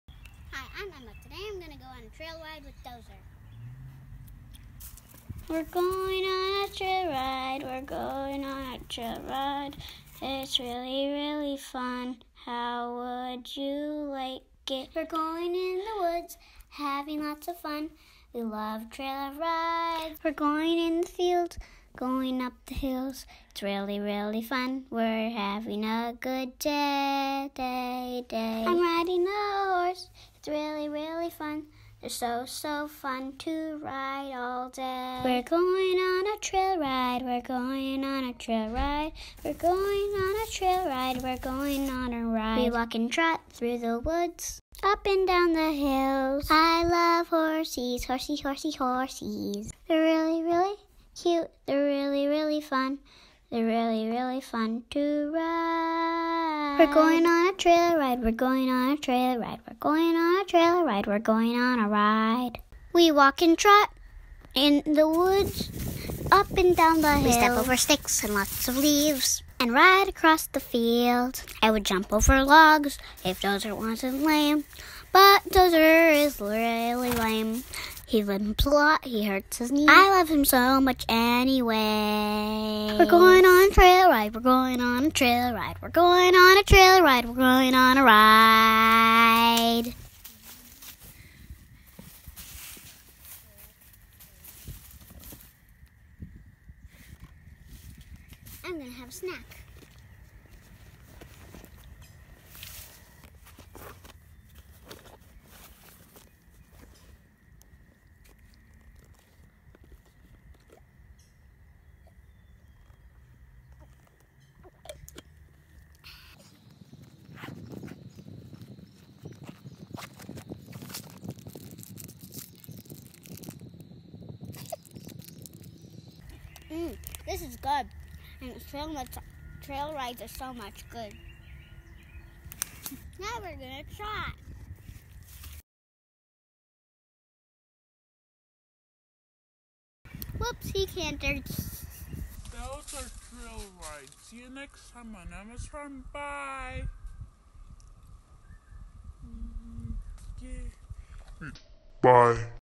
Trail ride with Dozer
Emma goes on a fun trail ride with dozer.